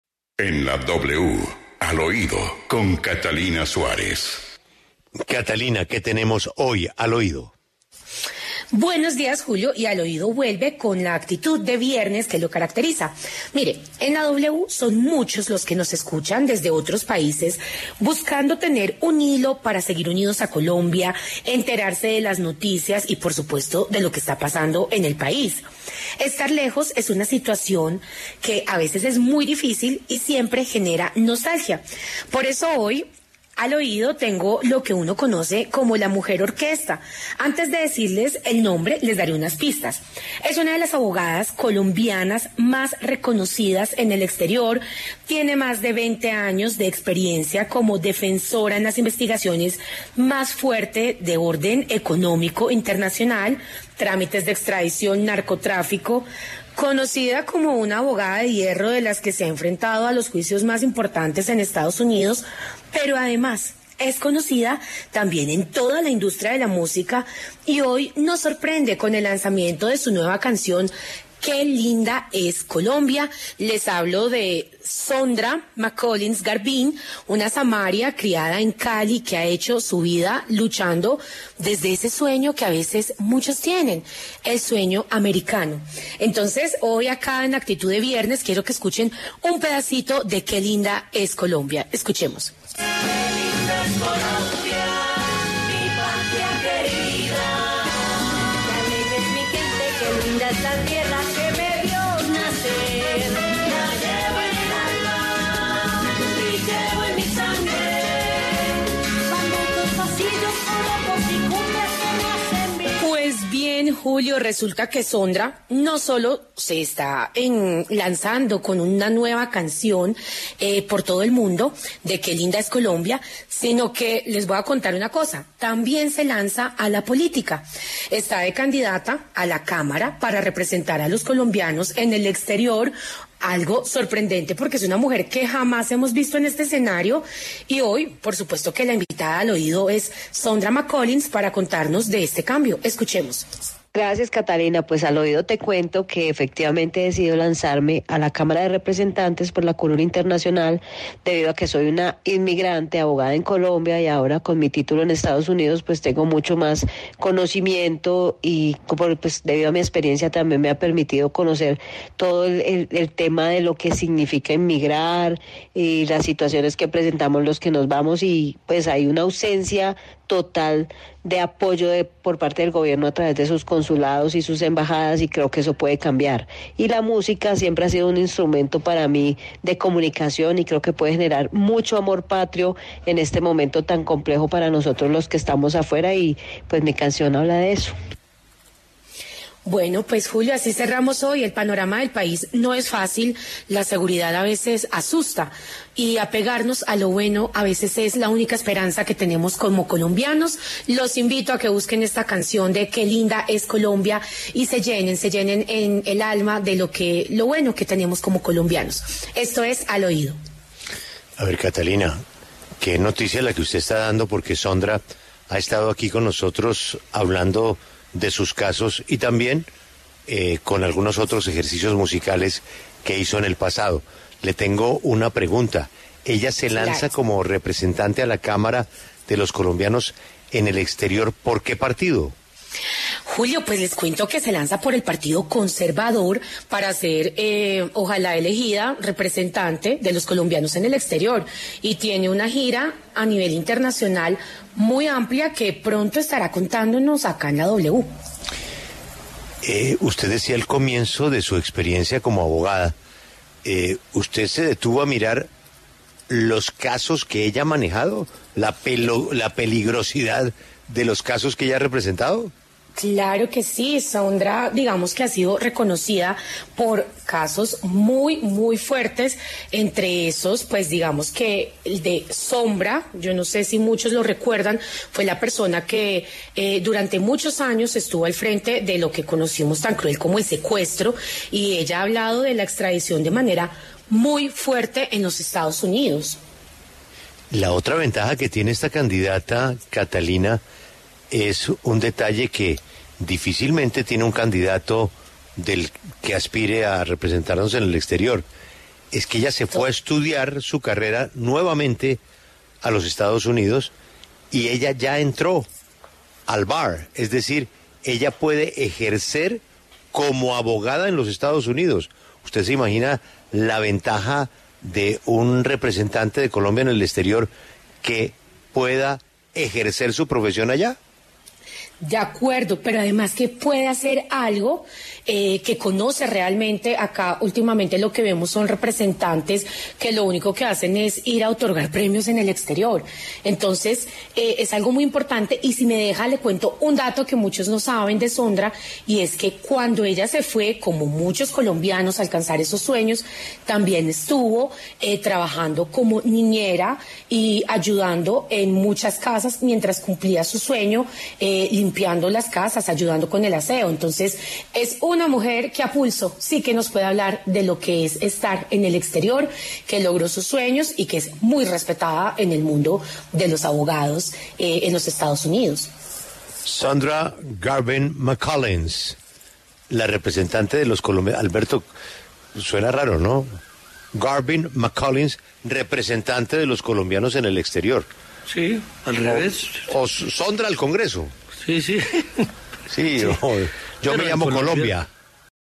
Sondra Macollins, la abogada de hierro, llega a la política y lanza un nuevo sencillo
Sondra Macollins Garvin tiene dos carreras exitosas: su vida de abogada internacional y su trayectoria como cantante.

Macollins es especializada en derecho penal y experta en trámites de extradición, es conocida en el mundo penal internacional como la abogada de hierro. Luego de ver desde su experiencia lo que sienten colombianos en diferentes países, decidió lanzarse por el Partido Conservador como candidata a la Cámara por colombianos en el exterior.